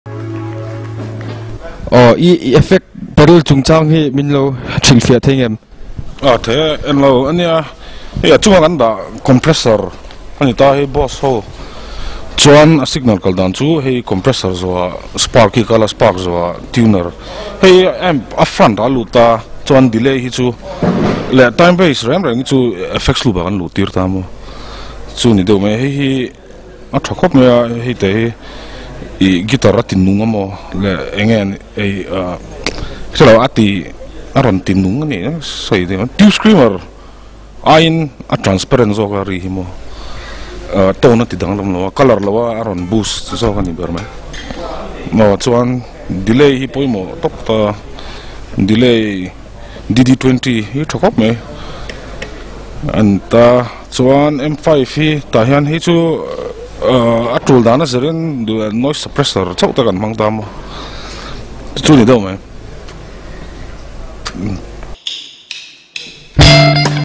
Toya Renthlei : Rig rundown @Concerto Mizoram - 5
Toya Renthlei hian Concerto Mizoram - 5 ah Instrumental mawi tak tak a rawn play ani.

Musicians :
Toya Renthlei - Lead Guitars
Bobo Renthlei - Rhythm Guitars
Tetea - Drums
Sangtea Ronald - Keyboard
Sangtea - Bass Guitar

Performed live on 28th March 2015@Vanapa Hall, Aizawl Mizoram.